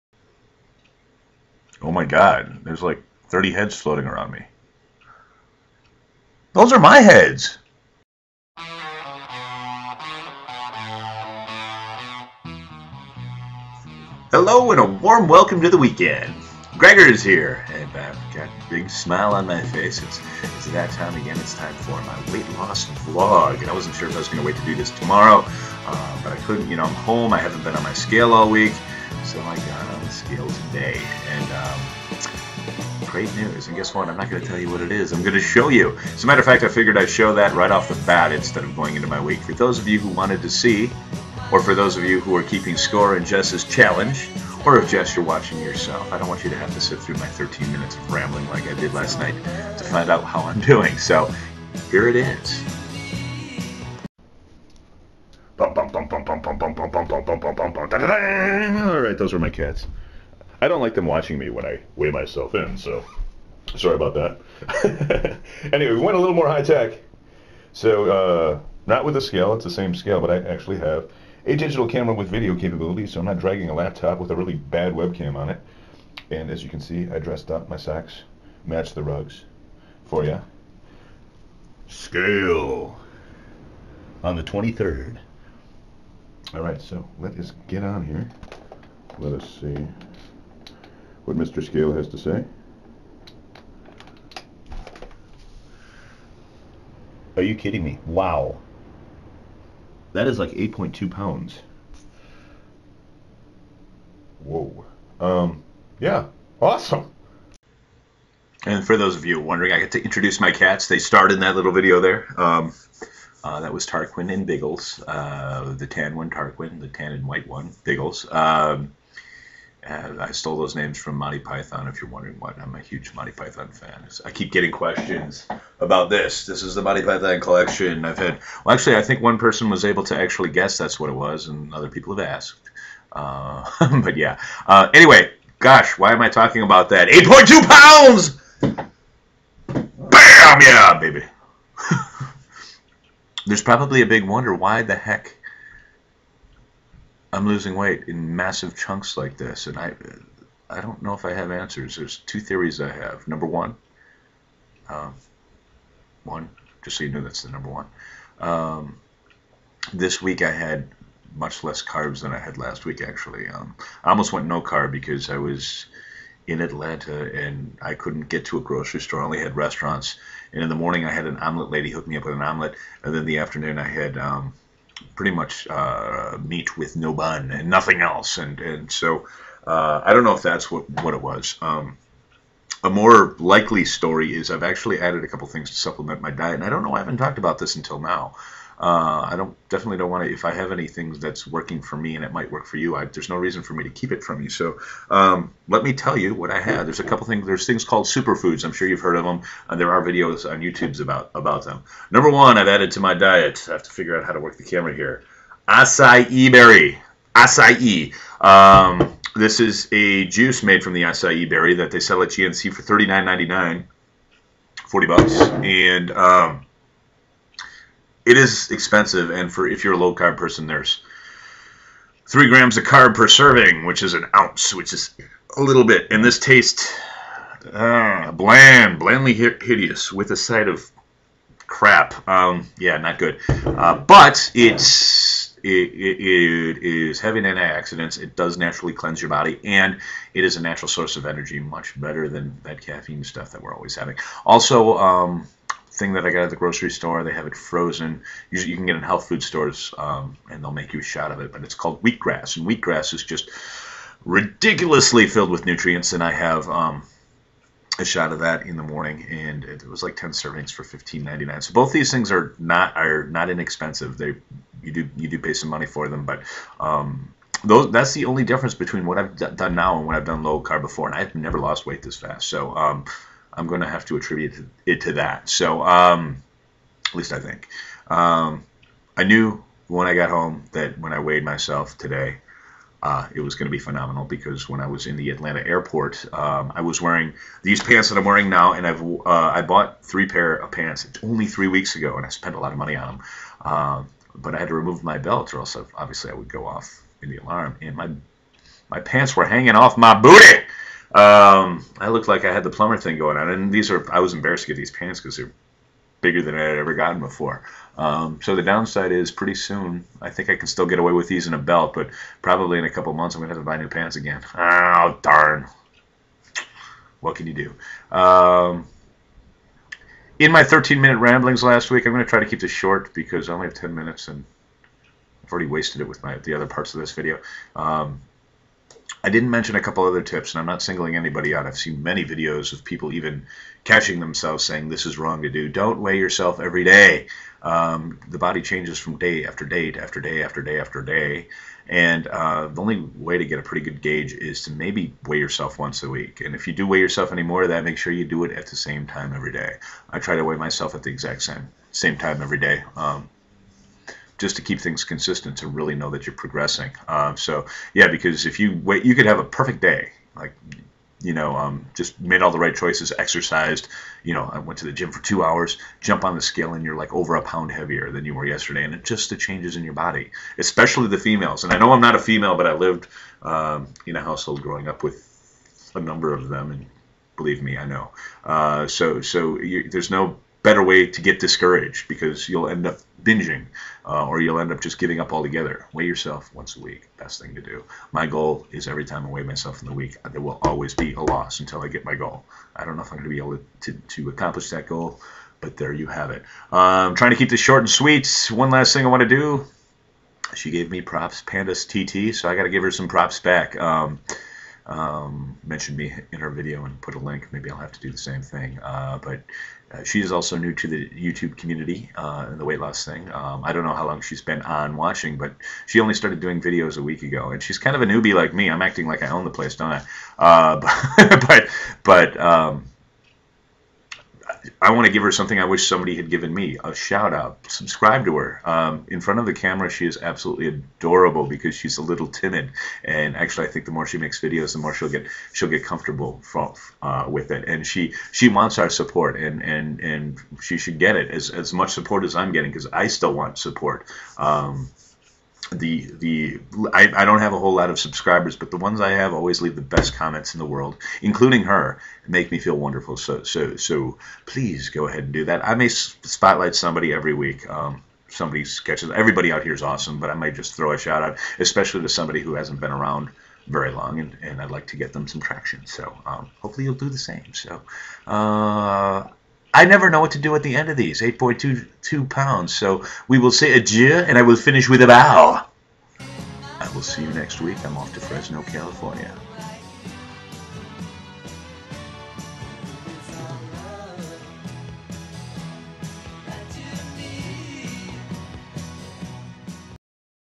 Weight Loss Vlog #3 - Take 2 - Weigh in Time
Ok, if you are wondering why the view count dropped and your comment is missing, this is a newer version of my vlog. I finally figured out how to get my new movie software to program properly to you tube (check out the picture in picture when I am weighing myself!!)
The lkast one was a lower quality version and I just can't have that :P

Oh yeah...I lost weight. lots of it!

Products I mentioned:
Acai Berry - you can get at any GNC
Wheat Grass - I got in my grcers freezer, but I don't see it in a lot of stores.